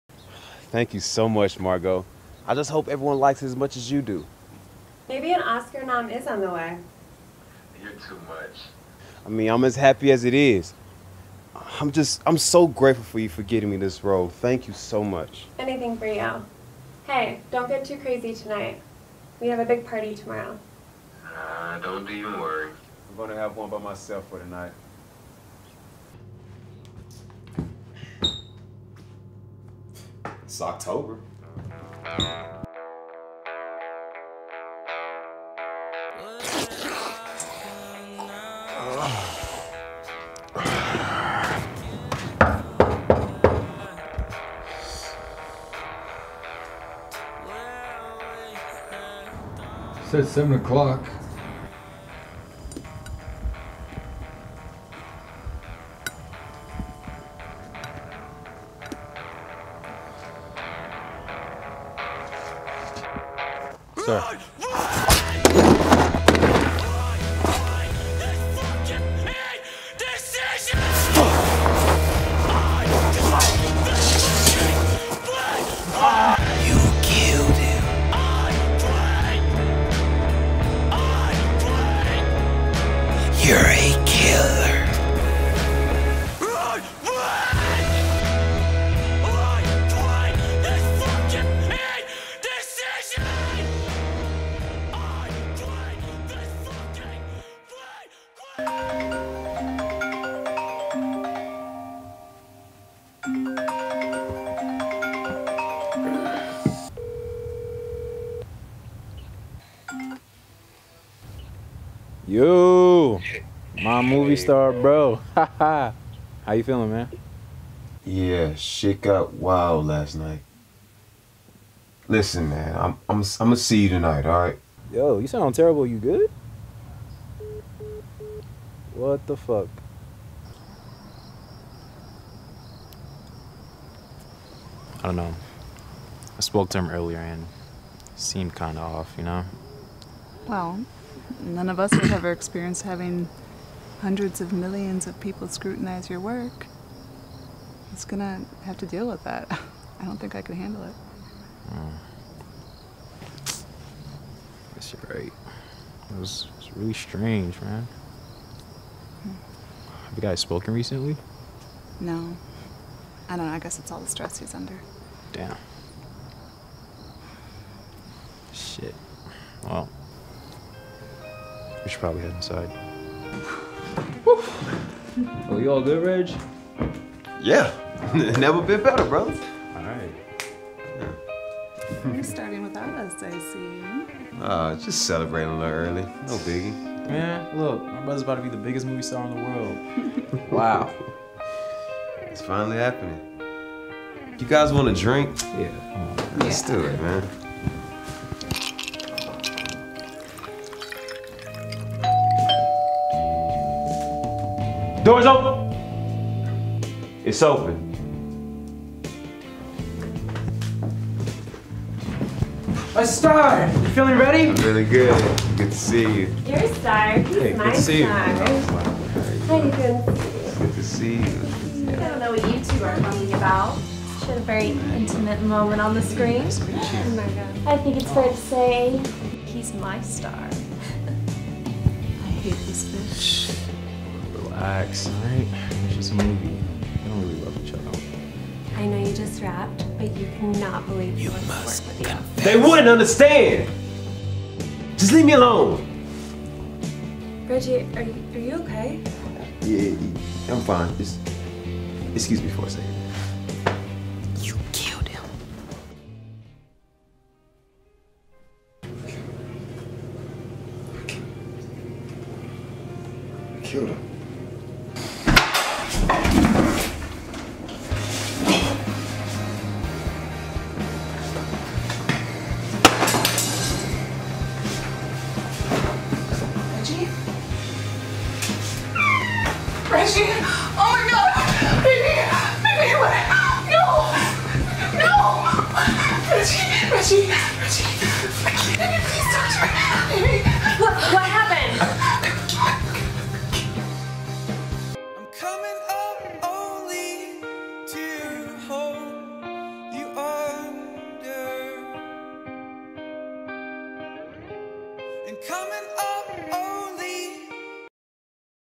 IN THE END
We had 120 hours to create a 5 minute horror short film for Playhouse West Film Festival. The logline is..."THE PUMPKIN KNOWS"

TEAM
Director: Joe
AD: David Cris
DP/Producer: Brendan Barlow
Sound: Randy Taylor
Writers: Joe & Natalie Ruxton
Costume Stylist: Sara Siegel
Editor: Kylie Judd

ACTORS (In order of film)
Reggie: Jaquevis Harmon
Margot: Sara Siegel
Plumber: Guy Reuven
Devon: Marcus Armstead
Amy: Megan Corse
Vincent: David Cris
Allegra: Kylie Judd